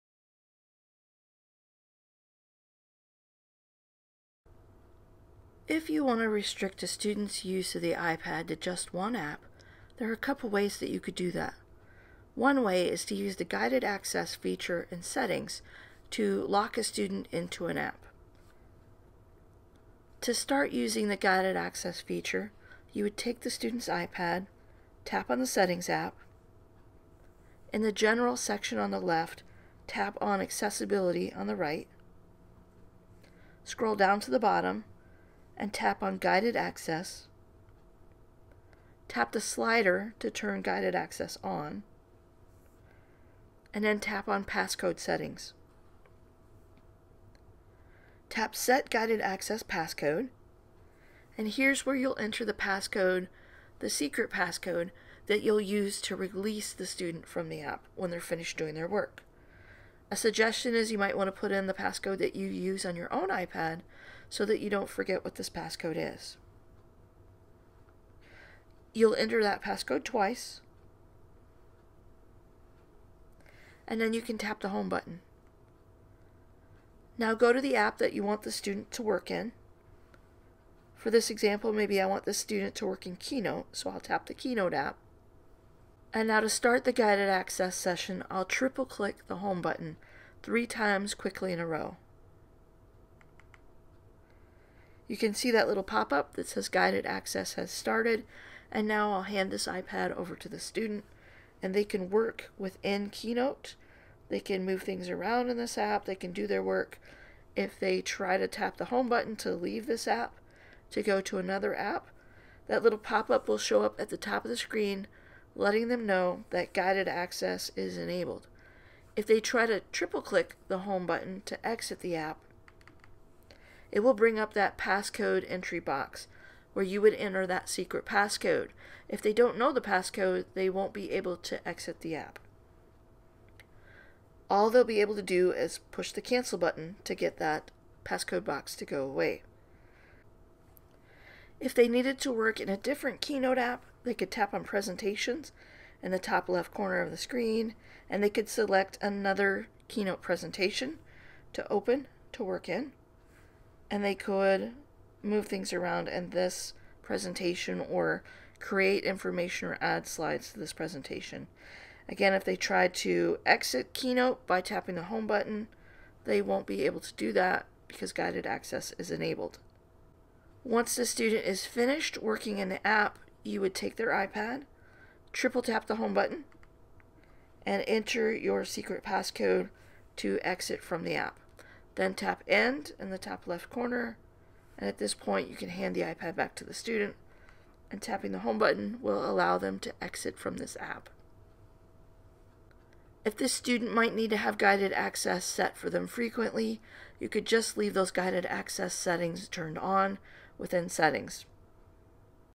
Enabling Guided Access On The iPad
How to enable the Guided Access accessibility feature on the iPad.